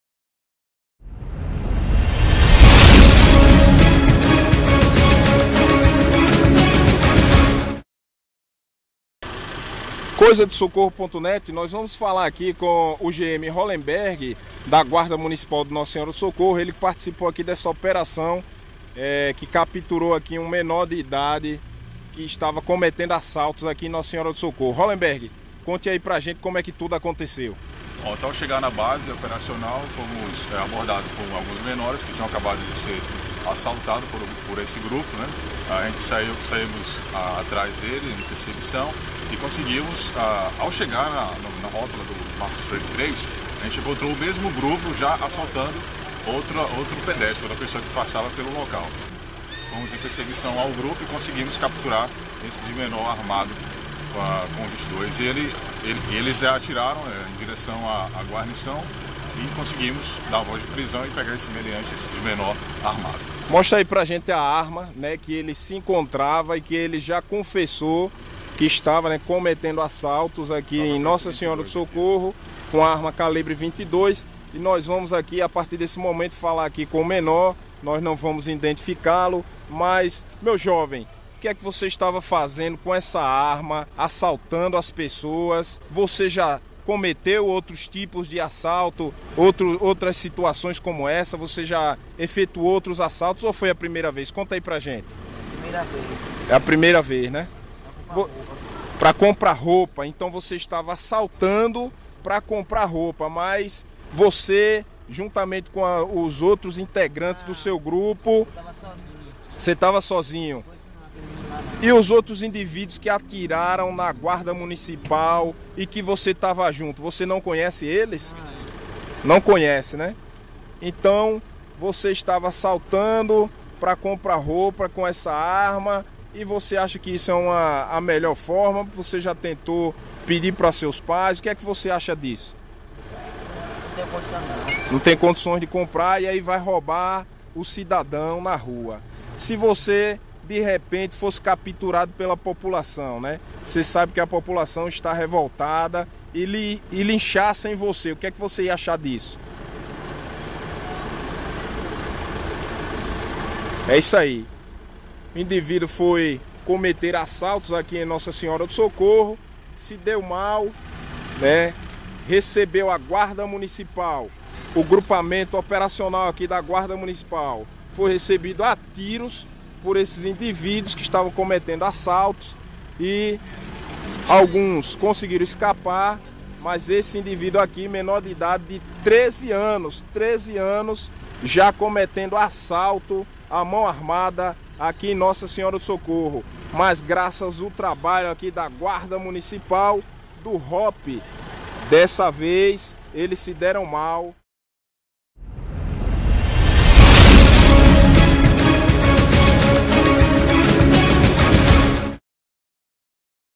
Guarda Municipal de Socorro é recebida a tiros após flagrar grupo efetuando assalto
A Guarda Municipal de Nossa Senhora do Socorro (GMNSS), apreendeu na noite deste sábado, (17/05), um menor de 13 anos que juntamente com cinco indivíduos, praticava assaltos a mão armada no entorno dos conjuntos Marcos Freire I e II.

Segundo informações da GM, um grupo de jovens solicitou o apoio dos Guardas após ser assaltado pelo bando, uma Guarnição da Ronda Ostensiva Preventiva Especializada (ROPE), iniciou as buscas e encontros os suspeitos efetuando um novo assalto. Os bandidos ao perceberem a aproximação da viatura efetuaram vários disparos contra os agentes e empreenderam fuga, felizmente nenhum Guarda se feriu.

Cinco assaltantes conseguiram escapar exceto I. C. S., de 13 anos, que foi apreendido e com ele foi encontrado um revolver calibre .22, o garoto assumiu que teria efetuado os assaltos para comprar roupa e que a arma foi comprada por ele na feira das trocas.

As vítimas que tiveram celulares e documentos roubados, disseram que os ladrões foram violentos e portavam além da arma apreendida uma faca e mais um revolver.

Após o flagrante o jovem foi encaminhado para a delegacia plantonista para adoção das medidas cabíveis.